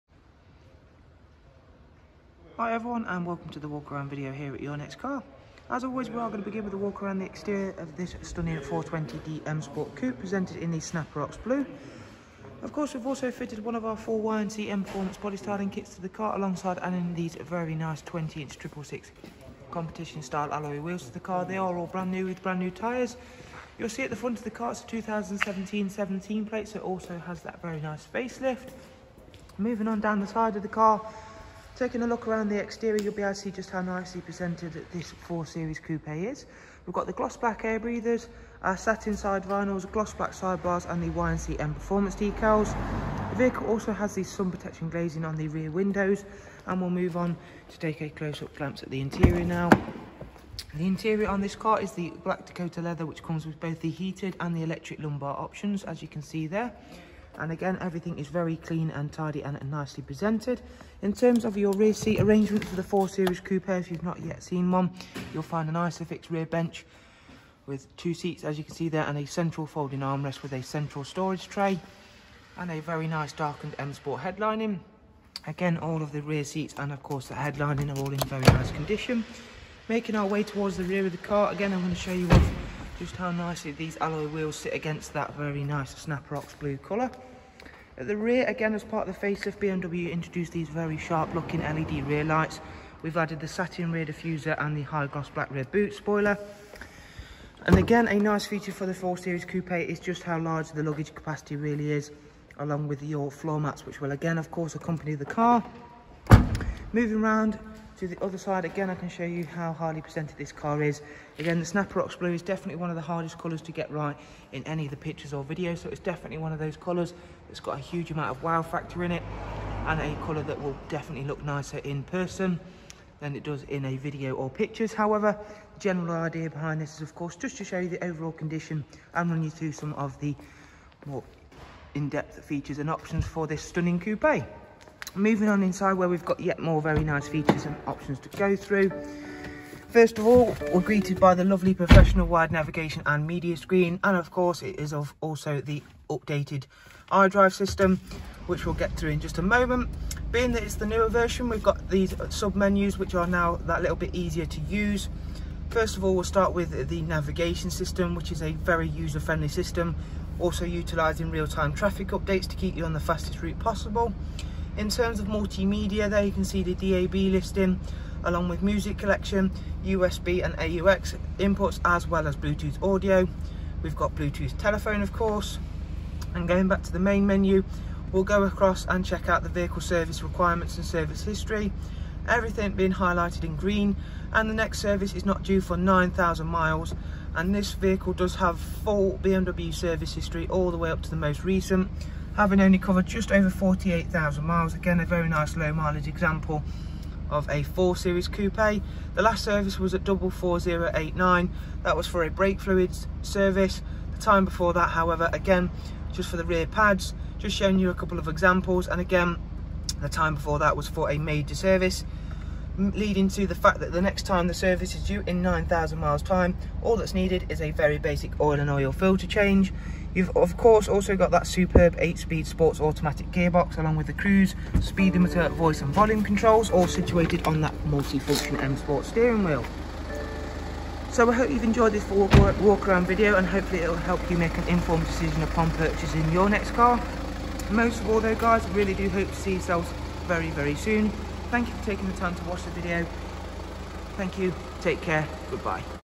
NEW IN AT YNC FACELIFT 420D COUPE! YE17YEK (Snapper Rock Blue!!)
- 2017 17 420D M Sport Coupe
- Full YNC Mperformance Body Styling Kit
- 20' 666 Competition Style Alloy Wheels
- Black Dakota Leather
- Full BMW Service History
- 8 Speed Sports Auto
- In Stock Ready To Go
Or Call On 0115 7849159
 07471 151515